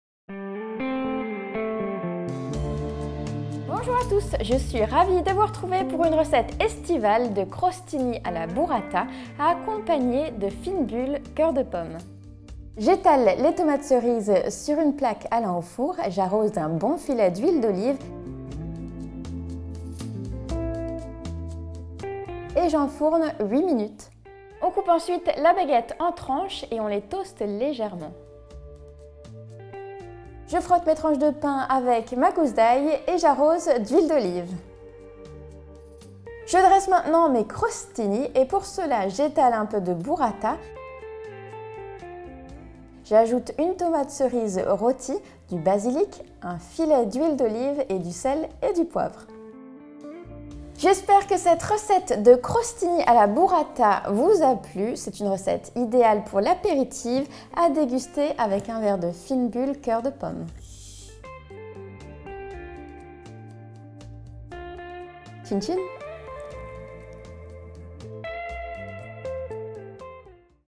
Recette : Crostini à la burrata et aux tomates cerises
Envie d'une recette facile et rapide pour un apéro d'été très gourmand ? Je vous propose mes crostini à la burrata. Un vrai délice à réaliser avec 3 fois rien et à déguster avec un bon verre de fines bulles à la pomme de chez Coeur de Pom'.

Toutes les recettes sont détaillées sur le blog.